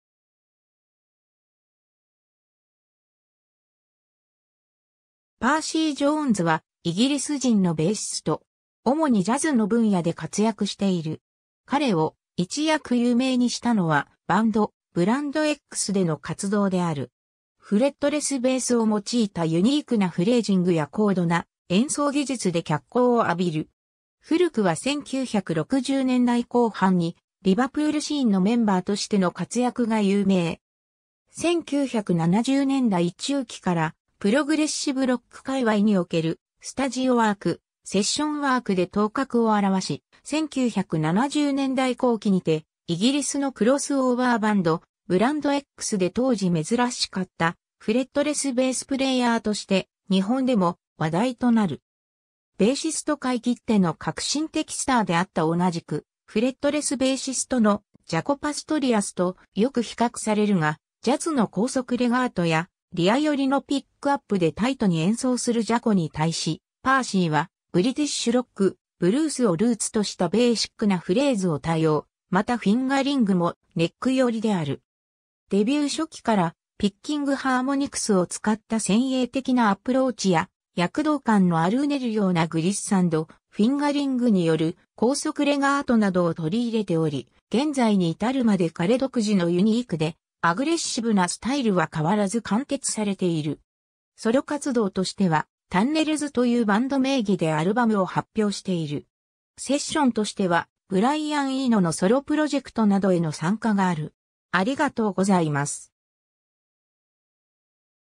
パーシー・ジョーンズ
フュージョン・ミュージシャン
パーシー・ジョーンズ（Percy Jones）は、イギリス人のベーシスト。
主にジャズの分野で活躍している。
彼を一躍有名にしたのはバンド、ブランドXでの活動である。
フレットレスベースを用いたユニークなフレージングや高度な演奏技術で脚光を浴びる。
 古くは1960年代後半に「Liverpool Scene」のメンバーとしての活躍が有名。
 1970年代中期からプログレッシブ・ロック界隈におけるスタジオワーク、セッションワークで頭角をあらわし、1970年代後期にてイギリスのクロスオーバー・バンド、ブランドXで当時めずらしかったフレットレスベース・プレイヤーとして日本でも話題となる。
 ベーシスト界きっての革新的スターであった同じくフレットレスベーシストのジャコ・パストリアスとよく比較されるが、ジャズの高速レガートやリア寄りのピックアップでタイトに演奏するジャコに対し、パーシーはブリティッシュ・ロック、ブルースをルーツとしたベーシックなフレーズを多用、またフィンガリングもネック寄りである。
 デビュー初期からピッキングハーモニクスを使った先鋭的なアプローチや、躍動感のあるうねるようなグリッサンド、フィンガリングによる高速レガートなどを取り入れており、現在に至るまで彼独自のユニークでアグレッシヴなスタイルは変わらず貫徹されている。
 ソロ活動としてはタンネルズというバンド名義でアルバムを発表している。
セッションとしてはブライアン・イーノのソロ・プロジェクトなどへの参加がある。